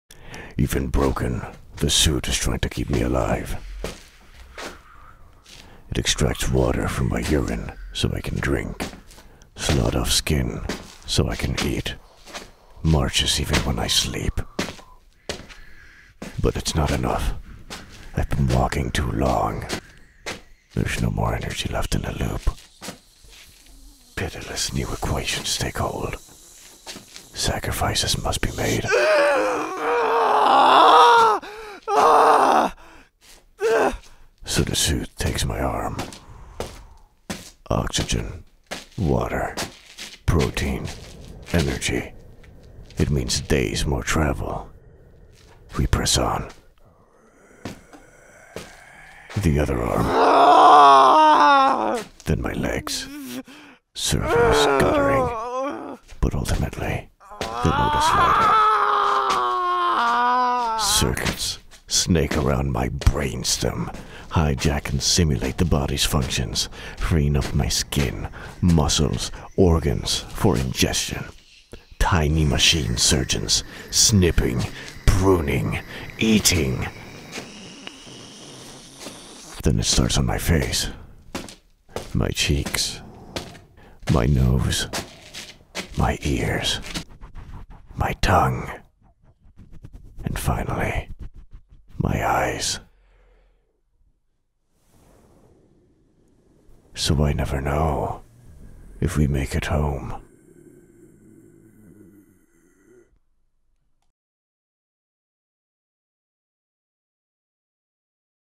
The Suit (No Music)| Badspacecomics Comic dub
Even broken the suit is trying to keep me alive. tiny machine surgeons snipping, pruning, eating